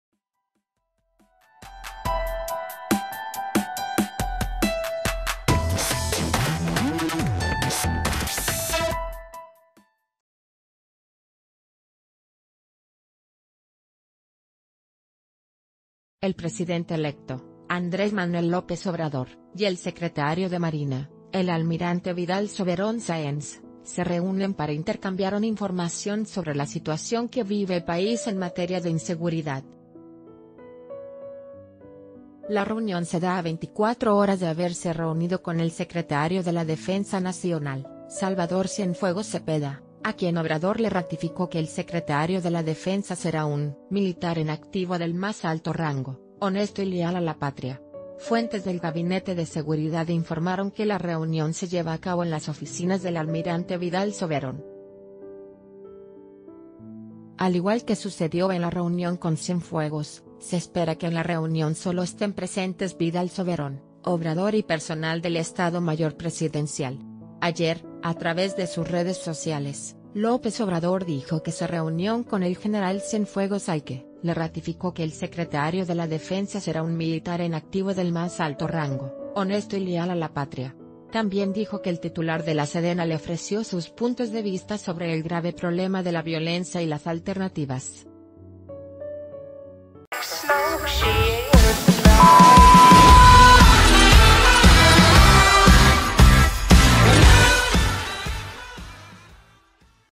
AMLO se reúne también con el secretario de Marina
El presidente electo, Andrés Manuel López Obrador, y el secretario de Marina, el almirante Vidal Soberón Saenz, se reúnen para intercambiaron información sobre la situación que vive el país en materia de inseguridad. La reunión se da a 24 horas de haberse reunido con el secretario de la Defensa Naci...